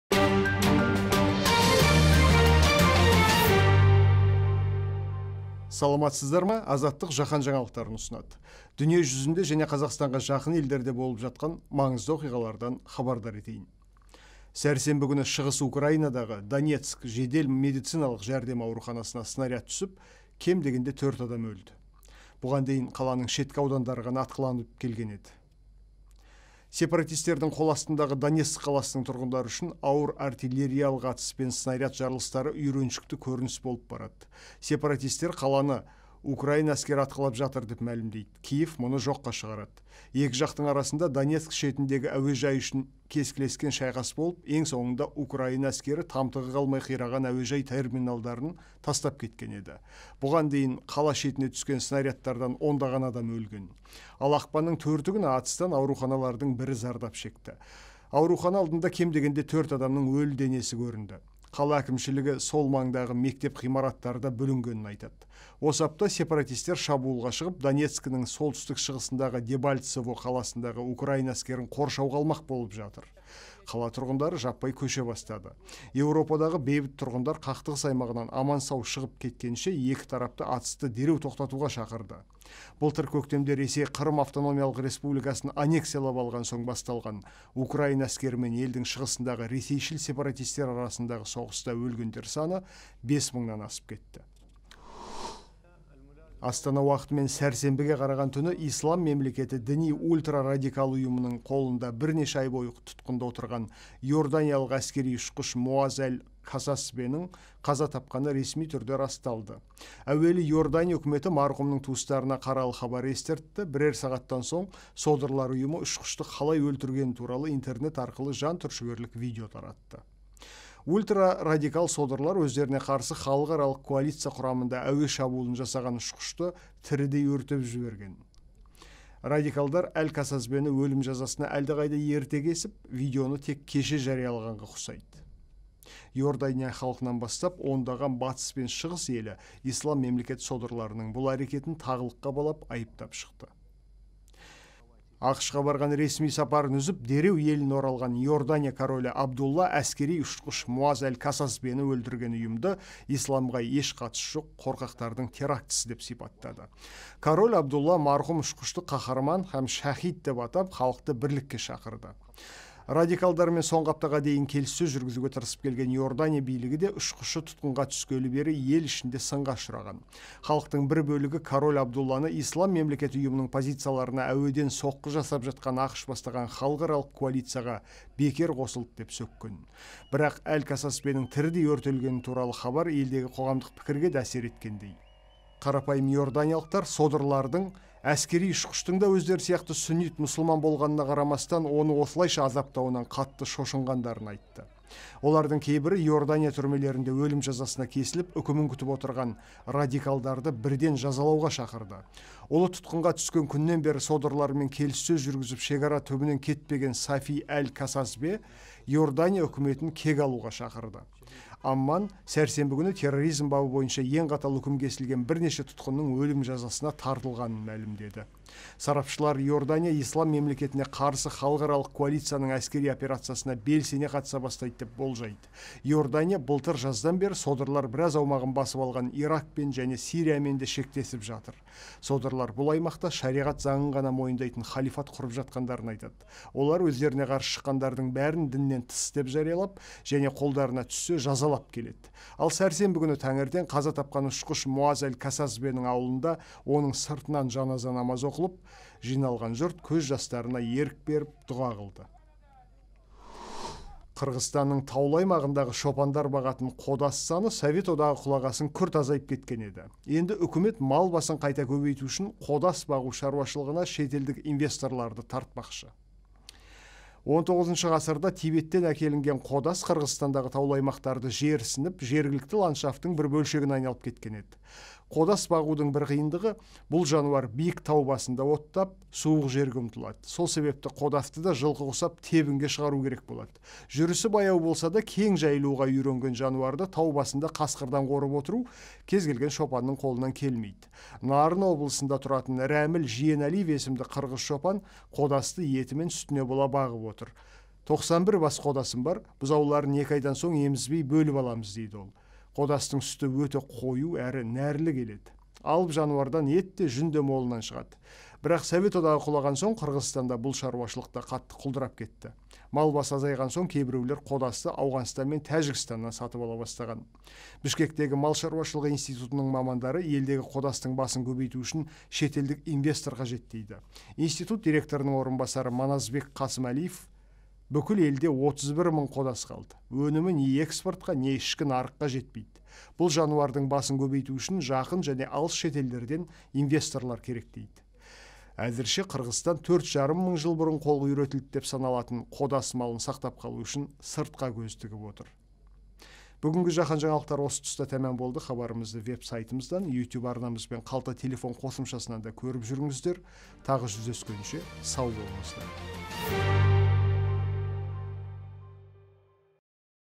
Жаһан жаңалықтары 04.02.2015
Донецк орталығындағы ауруханаға снаряд түсті, ИМ тірідей өртеген иорданиялық ұшқыштың әкесі кек қайтармақ, қырғыз қодасына инвестор қажет.